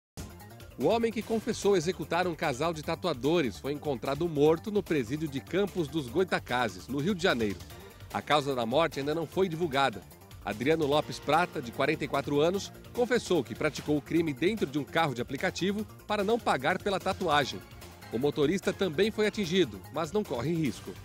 Homem que confessou executar casal de tatuadores é encontrado morto em presídio no RJ
O homem que confessou executar um casal de tatuadores foi encontrado morto no presídio de Campos dos Goytacazes, no Rio de Janeiro.